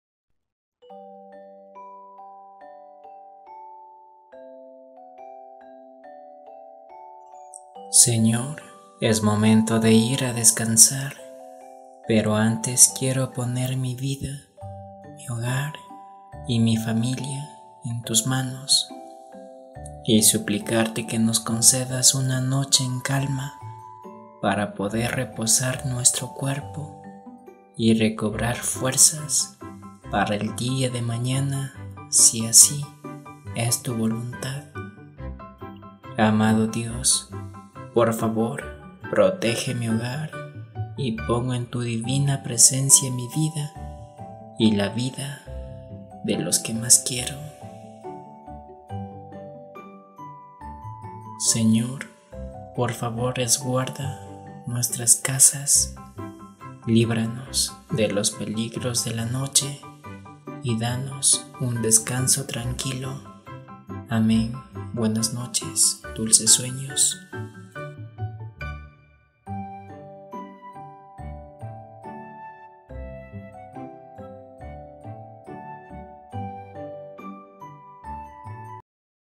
Buenas Noches Dules Sueños Abrelo Hermoso Mensaje Para Ti Que Dios Te Bendiga Feliz Noche
Mensaje de buenas noches para ti
Señor, es momento de ir a descansar, pero antes quiero poner mi vida